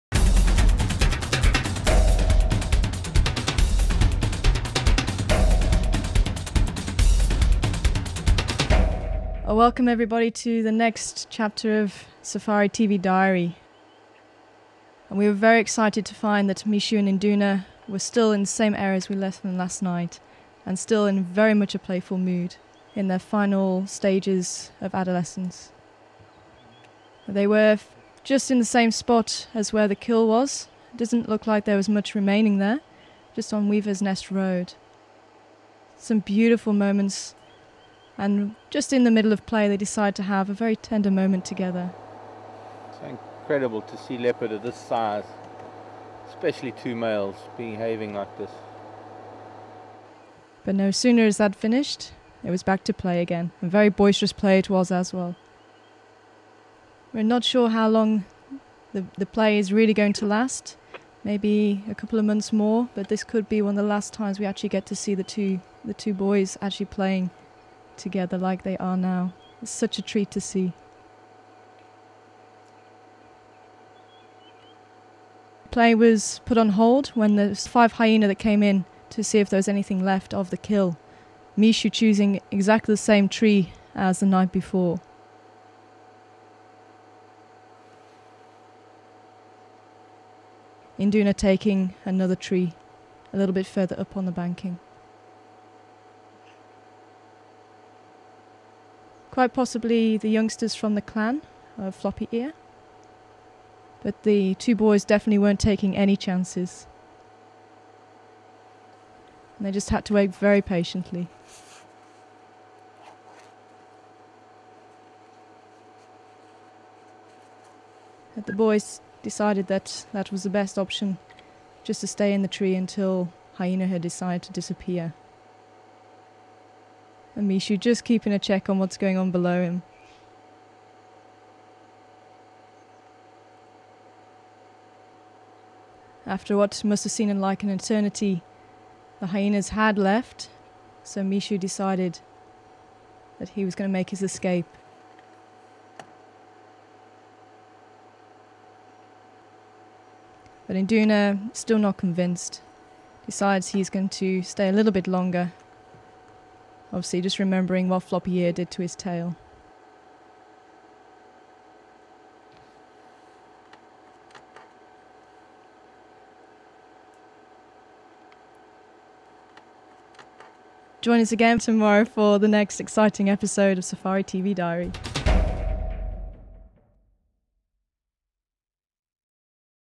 safari.tv diary - 009
The saga in yesterday's episode continues, with hyenas still trying to get at Mixo and Induna at the same spot, right after these leopards spend the entire morning in playful mood.